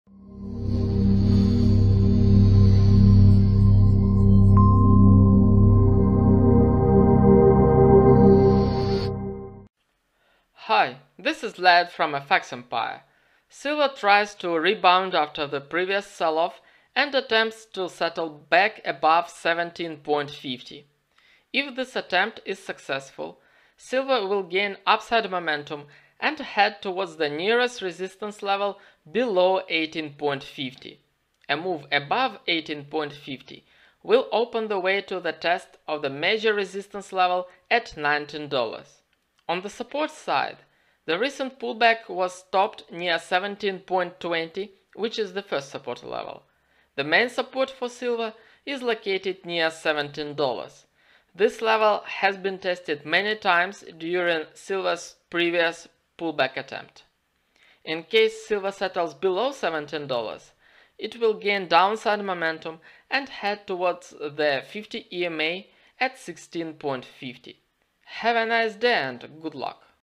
Silver Price Analysis For June 8, 2020 By FX Empire
Silver got support near $17.20 and tries to settle back above $17.50.